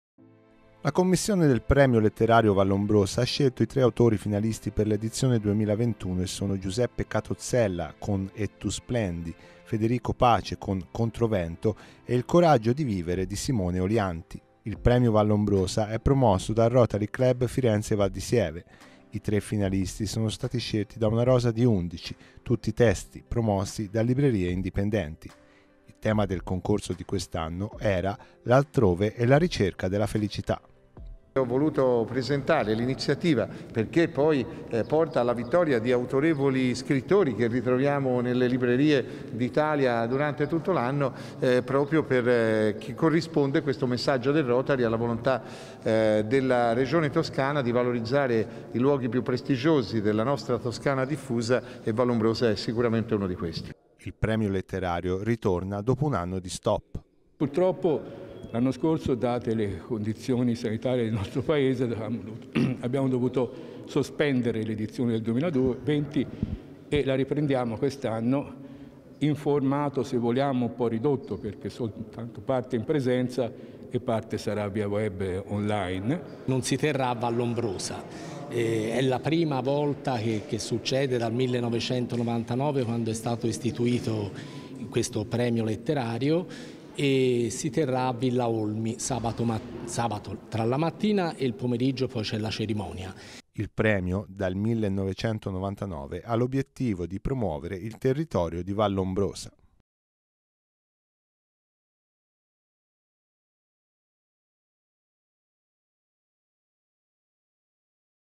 I tre finalisti del premio Vallombrosa
Sabato cinque giugno a Firenze appuntamento al resort Villa Olmi di Bagno a Ripoli con l'edizione 2020 / 21 del premio letterario Vallombrosa, che dal 1999 ferma la sua attenzione sulle opere legate ai territori della Toscana. “E tu splendi” di Giuseppe Catozzella, “Controvento” di Federico Pace e ‘’Il coraggio di vivere” di Simone Olianti, sono i tre libri finalisti, segnalati in una rosa di undici dalle librerie indipendenti fiorentine Alzaia, Libreria dei lettori e L’Orablu, in una 'edizione del premio che quest'anno era dedicata al tema "L'altrove e la ricerca della felicità". Sullo stesso tema si sono misurati anche gli studenti del liceo Ernesto Balducci di Pontassieve ed il miglior svolgimento verrà selezionato e premiato in collegamento diretto via Zoom dalla medesima giuria che esamina le opere letterarie. La notizia